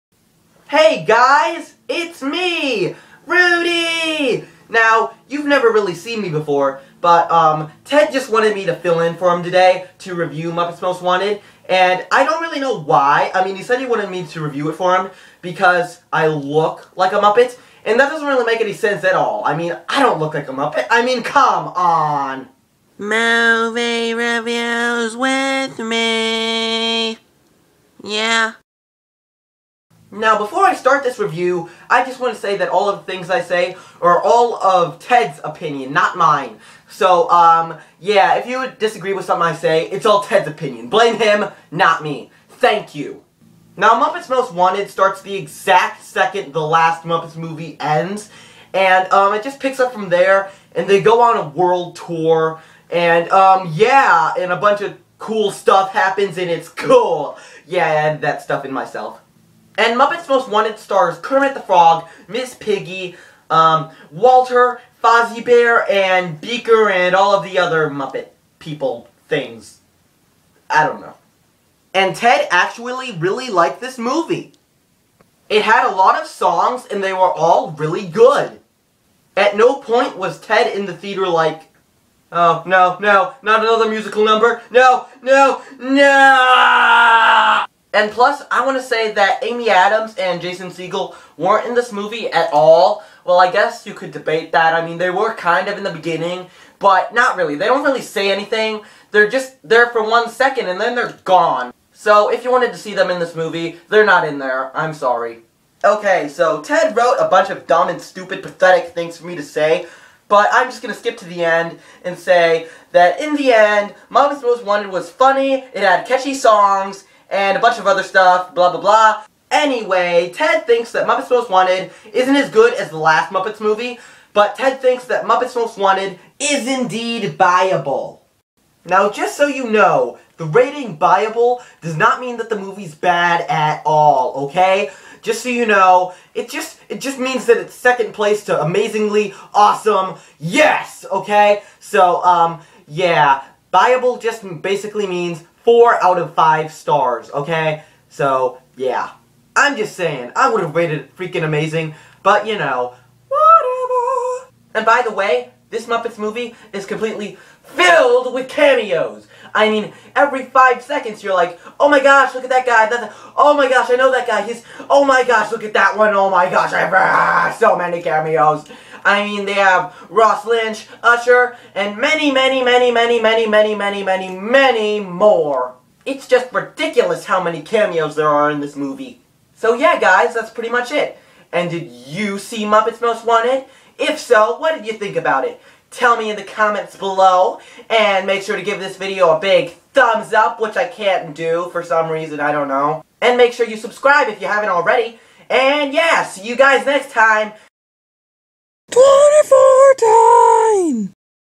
Muppets Most Wanted - Funny Movie Review
Kermit and the gang are back in the new Muppets sequel! Awesome? Failure? Ted, *Ahem!* I mean RUDY reviews Muppets Most Wanted!